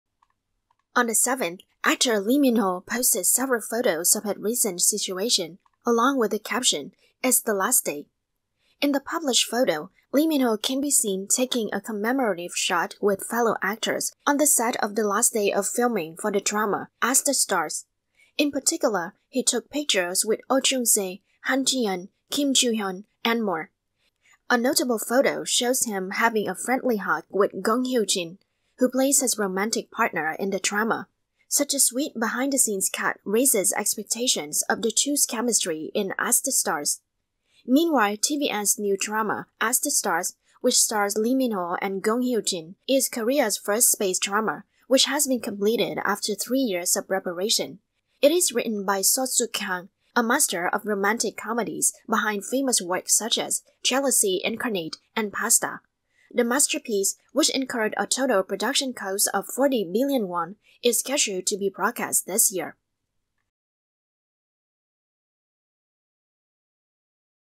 Lee Min Ho boasts fond hug with Gong Hyo Jin, is Kevin Oh jealous?
Actor Lee Min Ho recently flaunted his remarkable closeness with acting partner Gong Hyo Jin